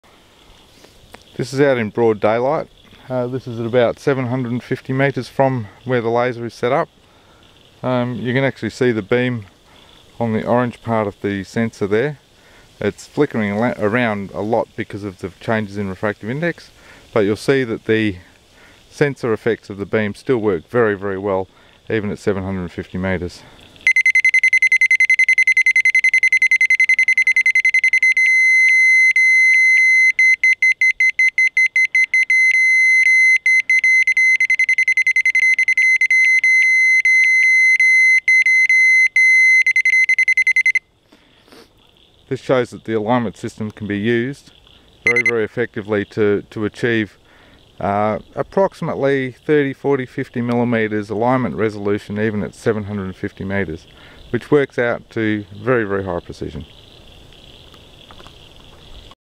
750m sensor
BLX-635 with sensing functions tested at 750 metres.